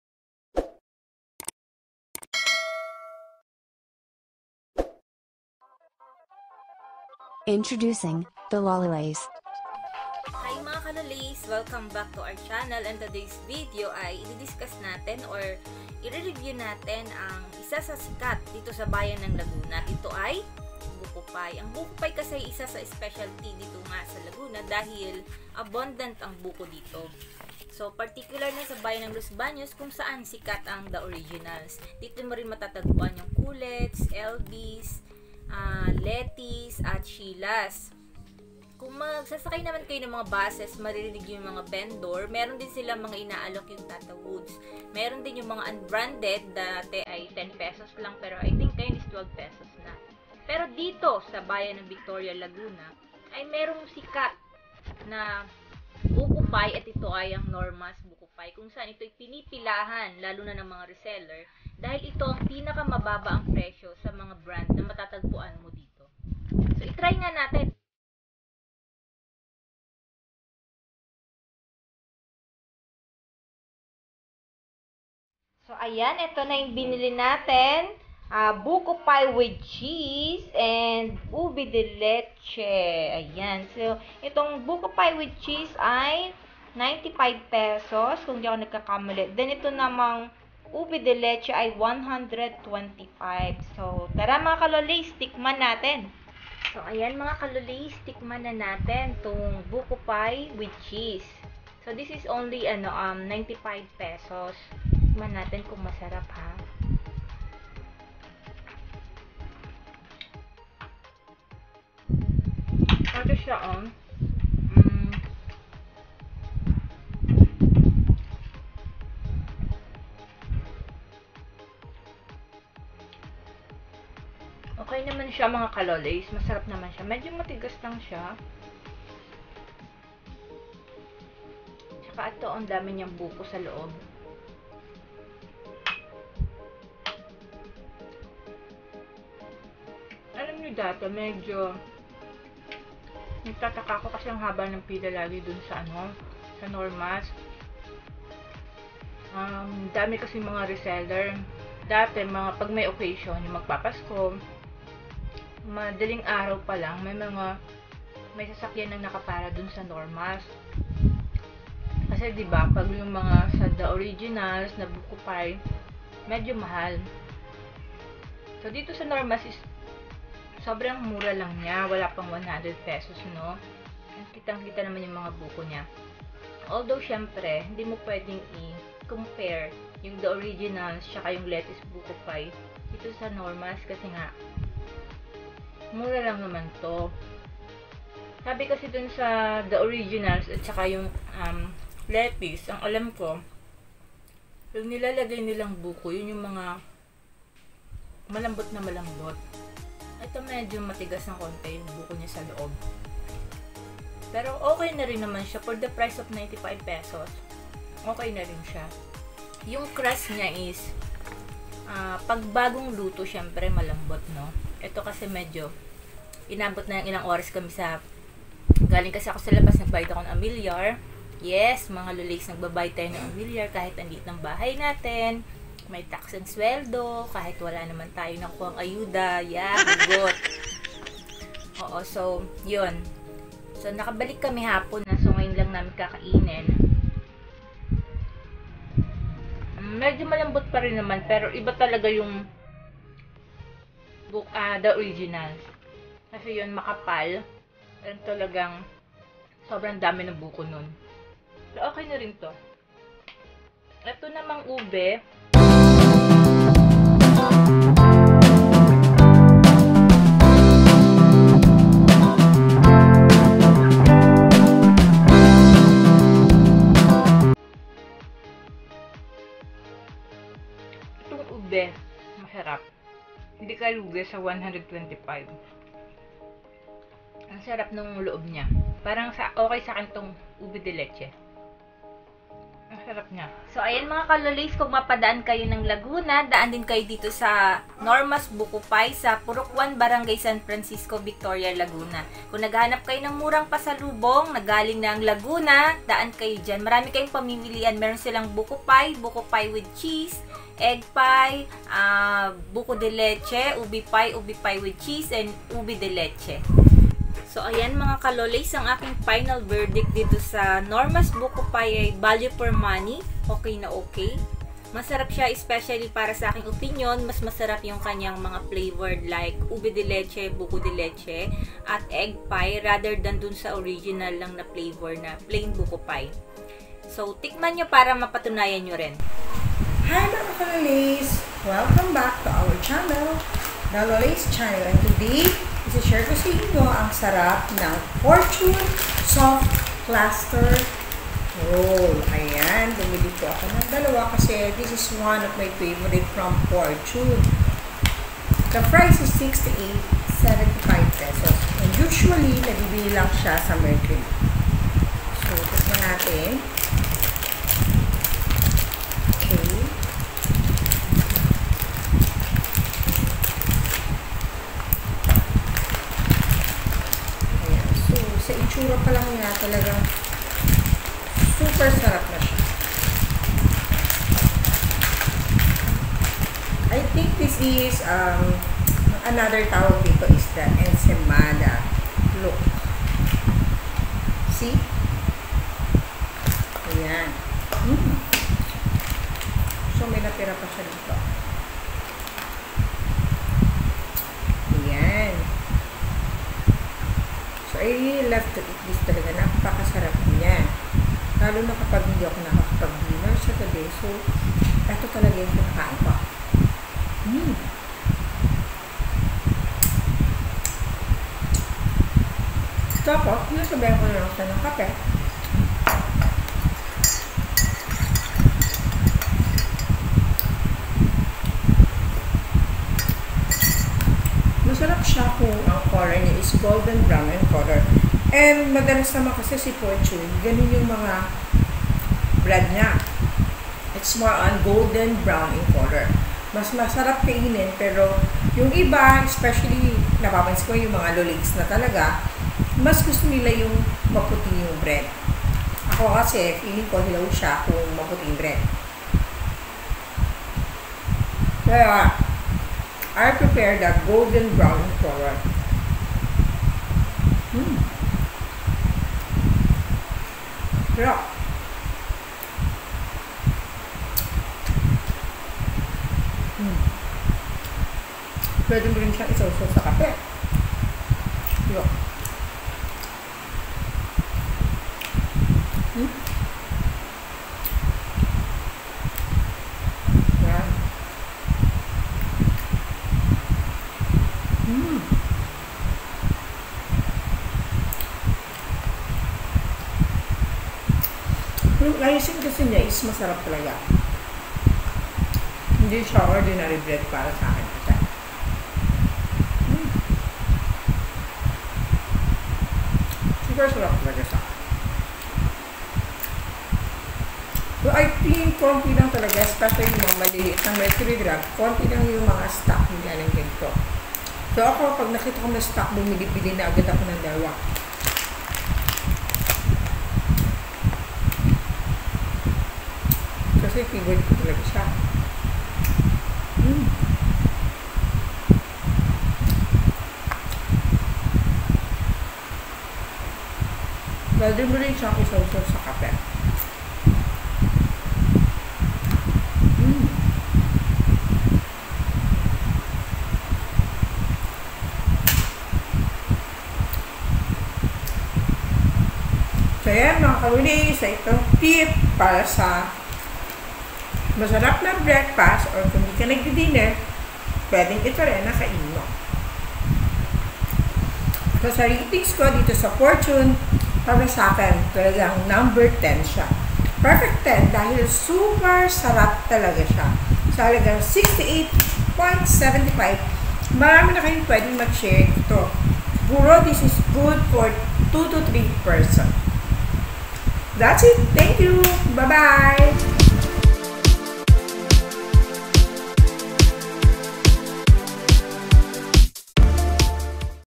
NORMA'S BUKO PIE & FORTUNE SOFT CLUSTER ROLL (A BACK TO BACK LOLEY'S FOOD REVIEW)
MURANG BUKO PIE AT MASARAP NA ENSAYMADA